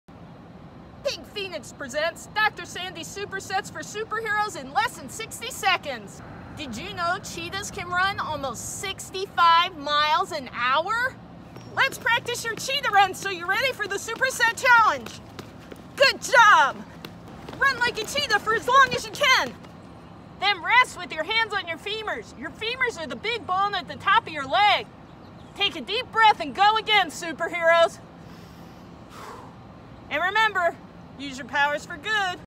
Cheetah Run!
Let's Cheetah Run for today's Superset Challenge!

Win a prize! Show us your Beast Moves! Post your picture completing the daily superhero challenge in the comments. One superhero each week will get a prize!

Remember Use Your Powers For Good!